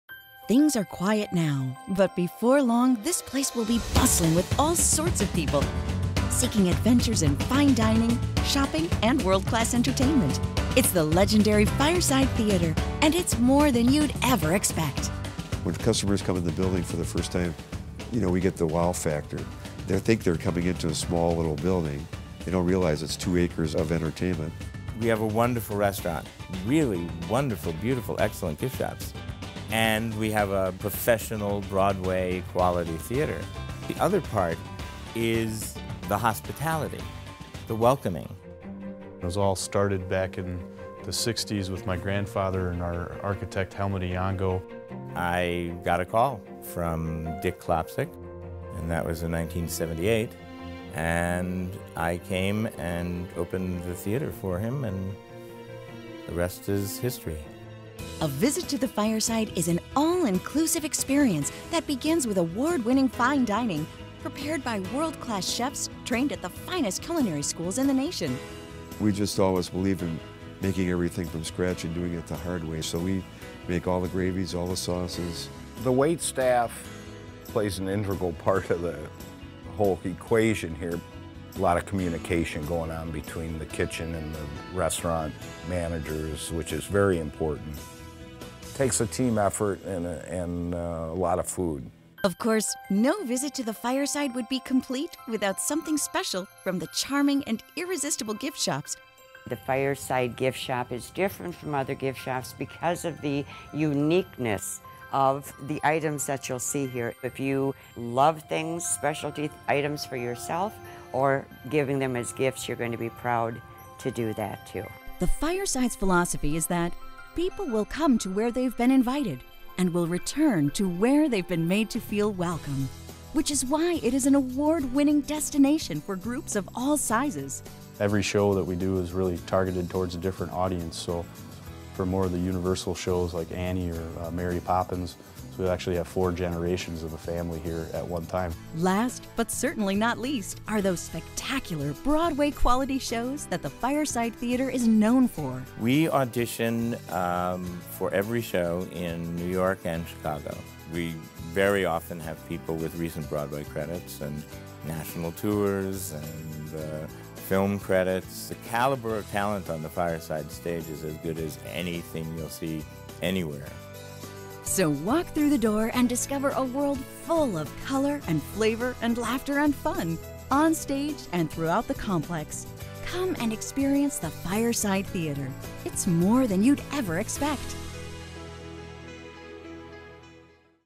The Fireside Theatre Facility Video
The Fireside Dinner Theatre is one of the Midwest’s most popular entertainment destinations and the only Actor’s Equity dinner theatre in Wisconsin. The Klopcic family is proud to celebrate over 54 years of hospitality in charming, historic Fort Atkinson. The Fireside Dinner Theatre, a favorite destination for couples, families and tour groups alike offers a variety of things to do.

We invite you to begin your Fireside experience by browsing through our spectacular gift shops or relaxing in the lounge with a refreshing treat from our menu of regular and alcohol free specialty drinks.  Then we will treat you to a superb dining experience in one of our lovely dining rooms.  Finally, enjoy a live professional show in our intimate theatre-in-the-round.  We hope your visit is an unforgettable adventure filled with more wonder, imagination and inspiration than you’d ever expect.